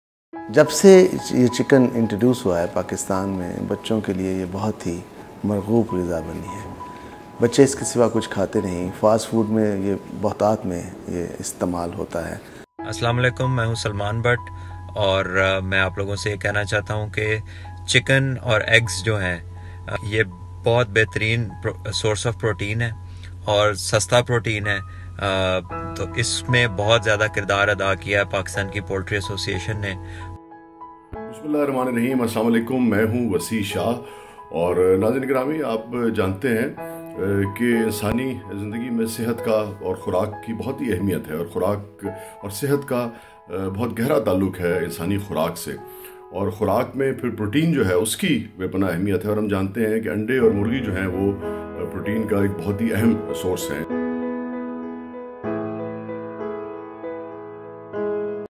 Syed Noor: Chicken is the Most Favorite of Children, No Doubt
Syed Noor along with Salman Butt and Wasi Shah raising awareness on the benefits of eggs and chicken consumption in human life.